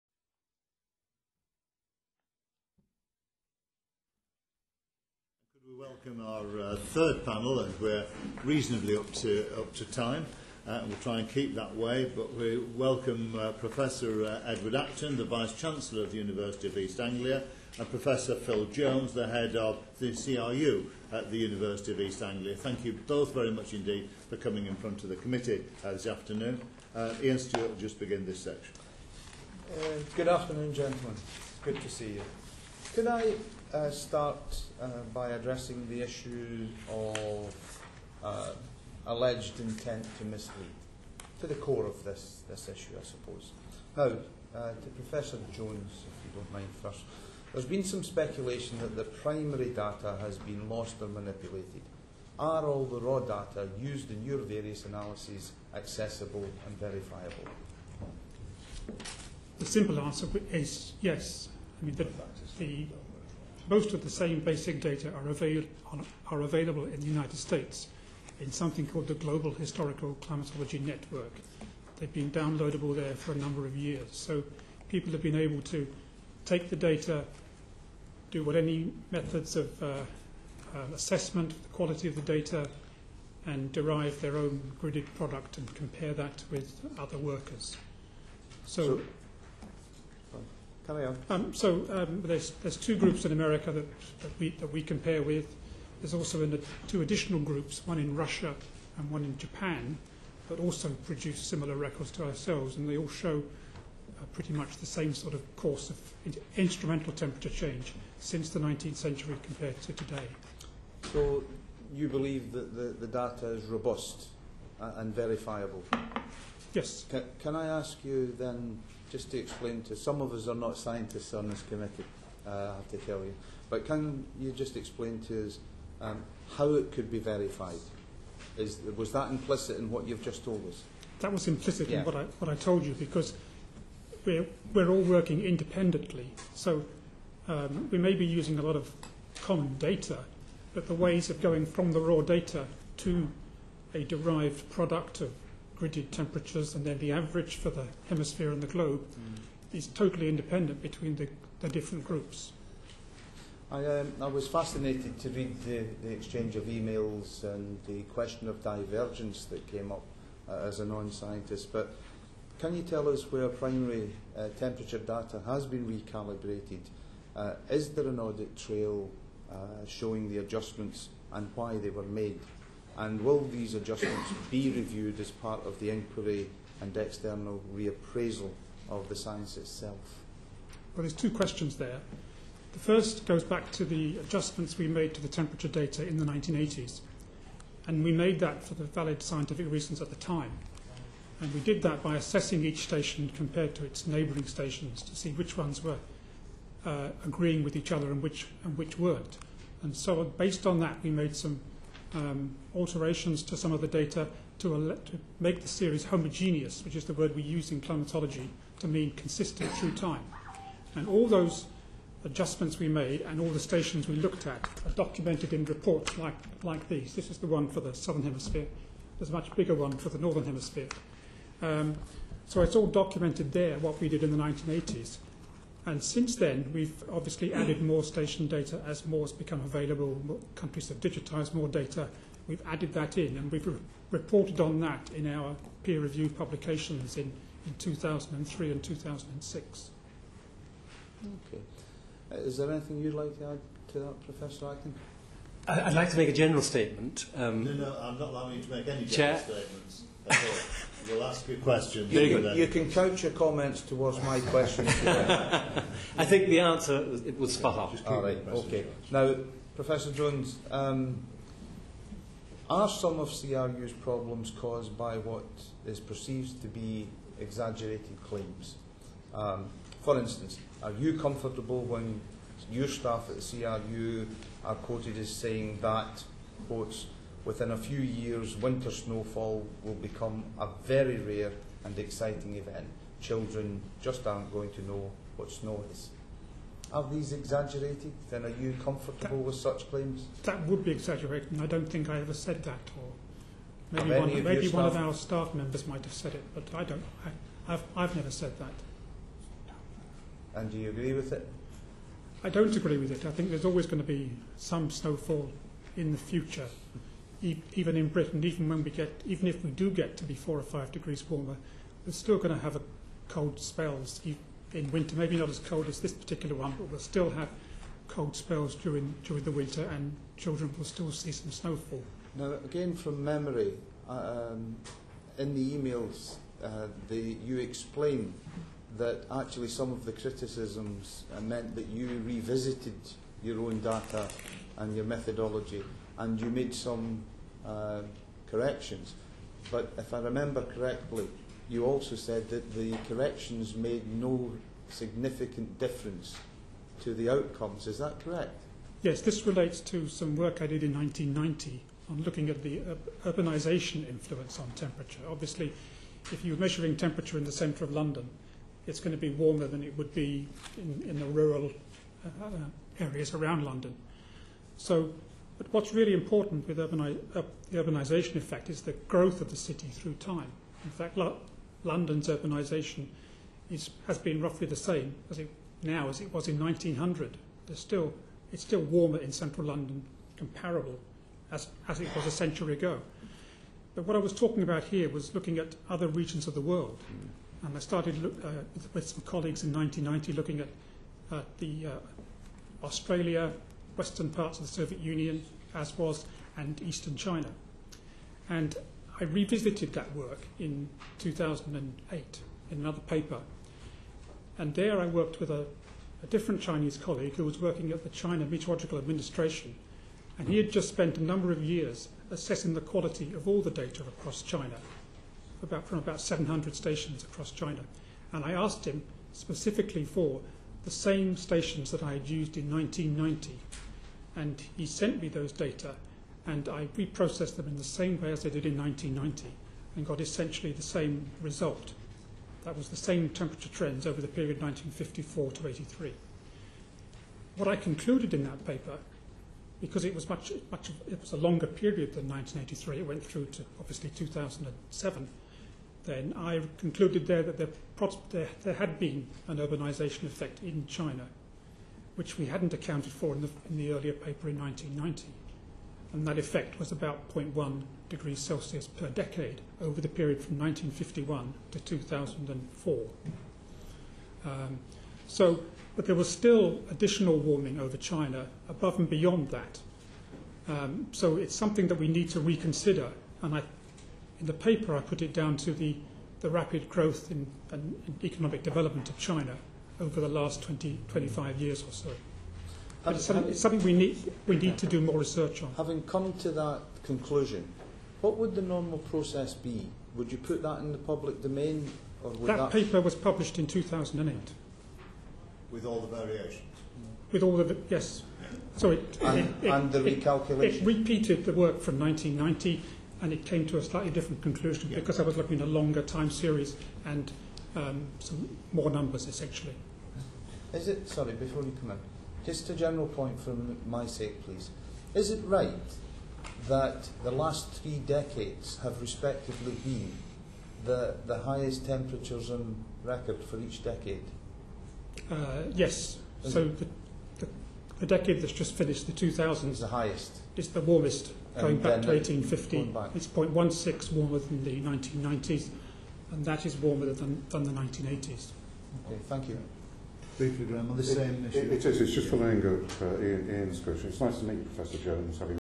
Phil Jones at the UK Parliament, 1 March 2010 [1/5]
Unedited video of Phil Jones' appearance at the UK Parliamentary Science and Technology Committee, with Vice Chancellor of the University of East Anglia, Prof Edward Acton.